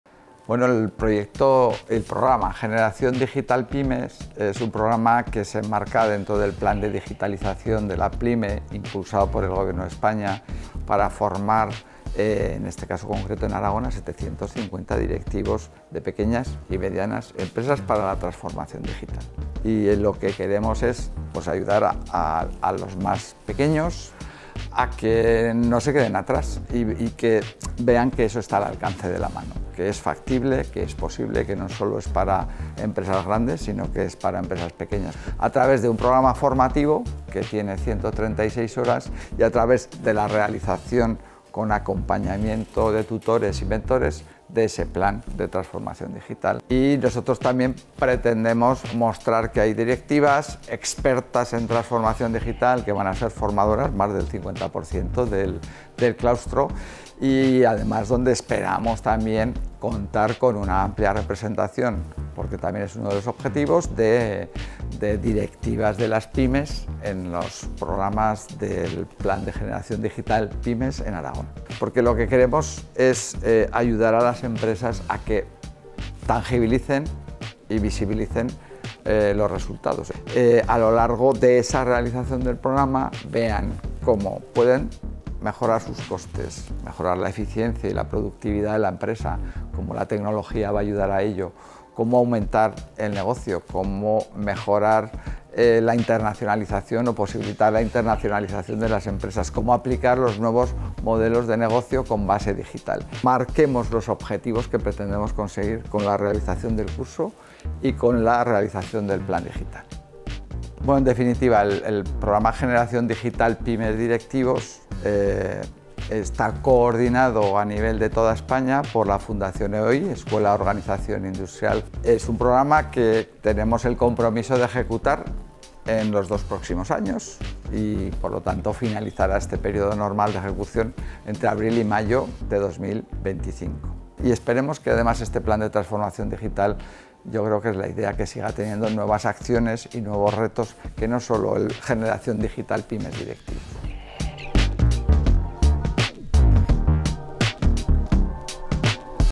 CESTE presenta el Programa Generación Digital PYMES-Directivos
CESTE, la escuela internacional de negocios con más de 30 años de experiencia en el mundo de la docencia, es la encargada de formar a más de 700 directivos PYMES en los próximos dos años a través del Programa Generación Digital para este sector.

Manuel J. Badal, director académico, lo cuenta en este 'Nos Presenta'